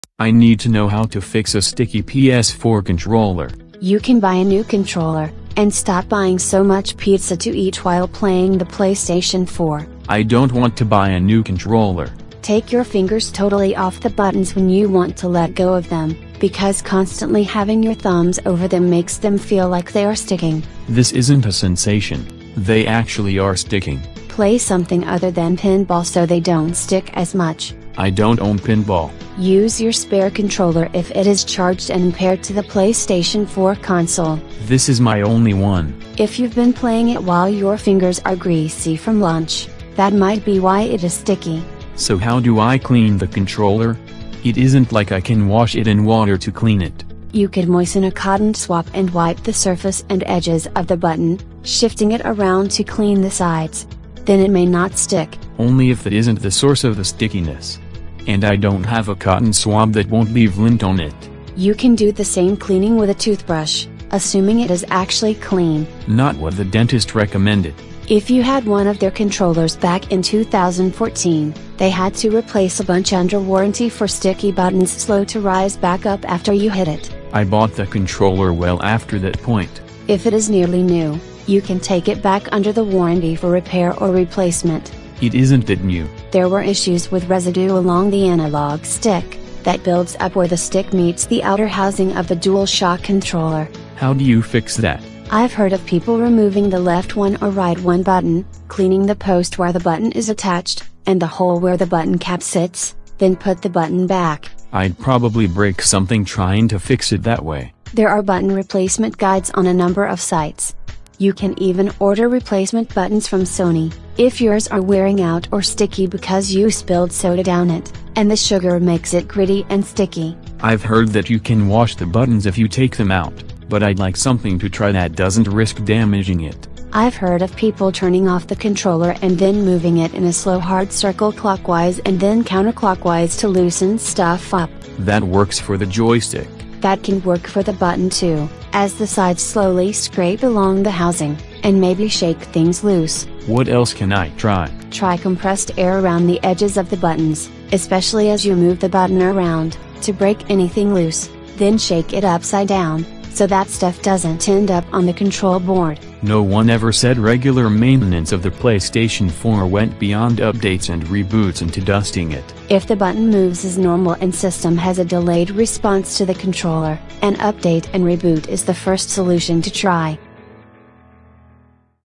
How to Fix a Sticky PS4 Controller
I need to know how to fix a sticky PS4 controller.

You can buy a new controller and stop buying so much pizza to eat while playing the Playstation 4.

I don't want to buy a new controller.

Take your fingers totally off the buttons when you want to let go of them, because constantly having your thumbs over them makes them feel like they are sticking.

This isn't a sensation, they actually are sticking.

Play something other than pinball so they don't stick as much.

I don't own pinball.

Use your spare controller if it is charged and paired to the PlayStation 4 console.

This is my only one.

If you've been playing it while your fingers are greasy from lunch, that might be why it is sticky.

So how do I clean the controller? It isn't like I can wash it in water to clean it.

You could moisten a cotton swap and wipe the surface and edges of the button, shifting it around to clean the sides. Then it may not stick.

Only if that isn't the source of the stickiness. And I don't have a cotton swab that won't leave lint on it.

You can do the same cleaning with a toothbrush, assuming it is actually clean.

Not what the dentist recommended.

If you had one of their controllers back in 2014, they had to replace a bunch under warranty for sticky buttons slow to rise back up after you hit it.

I bought the controller well after that point.

If it is nearly new, you can take it back under the warranty for repair or replacement.

It isn't that new.

There were issues with residue along the analog stick that builds up where the stick meets the outer housing of the Dual Shock controller.

How do you fix that?

I've heard of people removing the left one or right one button, cleaning the post where the button is attached and the hole where the button cap sits, then put the button back.

I'd probably break something trying to fix it that way.

There are button replacement guides on a number of sites. You can even order replacement buttons from Sony if yours are wearing out or sticky because you spilled soda down it and the sugar makes it gritty and sticky.

I've heard that you can wash the buttons if you take them out, but I'd like something to try that doesn’t risk damaging it.

I've heard of people turning off the controller and then moving it in a slow hard circle clockwise and then counter-clockwise to loosen stuff up.

That works for the joystick.

That can work for the button too, as the sides slowly scrape along the housing and maybe shake things loose.

What else can I try?

Try compressed air around the edges of the buttons, especially as you move the button around, to break anything loose, then shake it upside down so that stuff doesn't end up on the control board.

No one ever said regular maintenance of the PlayStation 4 went beyond updates and reboots into dusting it.

If the button moves as normal and system has a delayed response to the controller, an update and reboot is the first solution to try.